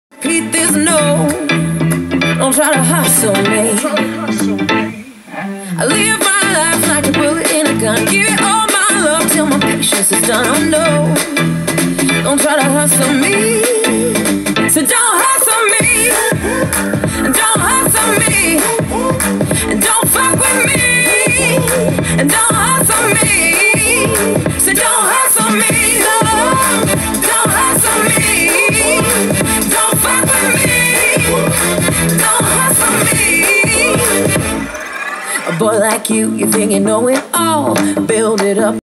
The 3cm Woofer "Little Fuckin' Wonder" flexing.
I spent a few days on this build. The internal guts are a mathematic secret of mine. The speaker had absolutely no power rating labelled on the mag, nor Ohm rating. It measure out to 5.5 ohms. I tried headphone power and it was too low and insufficient to get any volume. So I currently have it hooked up as a centre speaker in our surround sound. Here's the flex test. Decent bass response from the port.